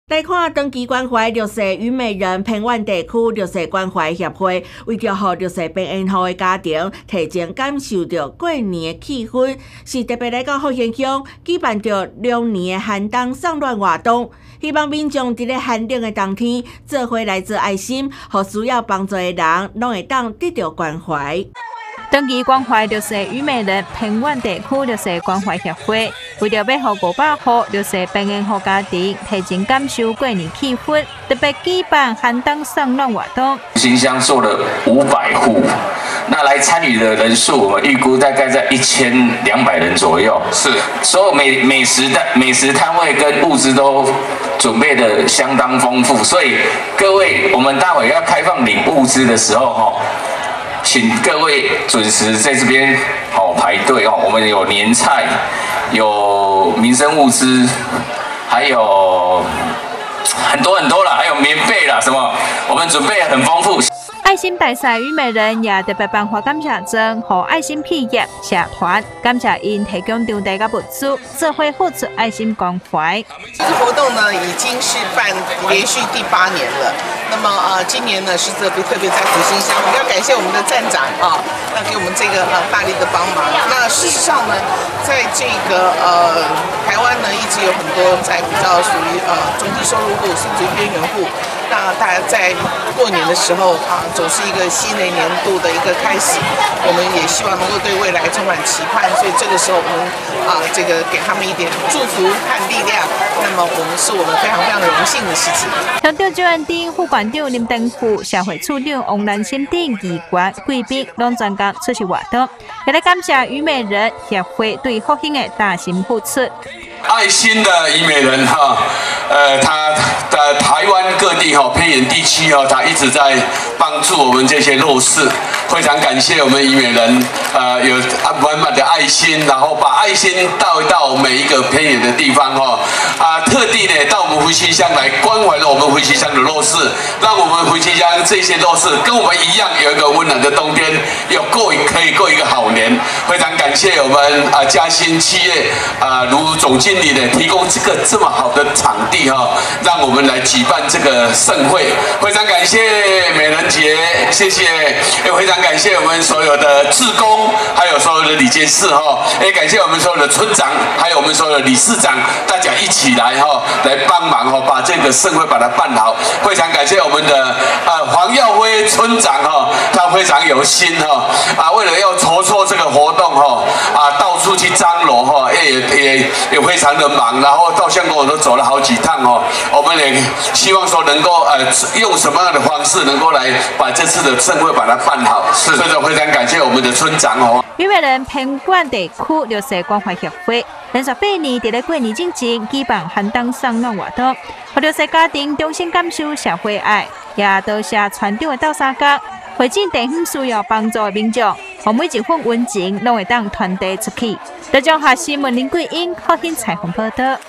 1130123 于美人寒冬送暖 協助500戶邊緣戶家庭幸福過年
藝人于美人與多個愛心企業、社團共同發起的2024龍行大運好運龍總來寒冬送暖活動，在福興鄉加興生化科技園區廣場溫馨登場，1800人500戶邊緣戶家庭，收到愛心民眾捐贈的年菜、米粉等物資，希望讓弱勢邊緣戶家庭能開心與家人圍爐過新年。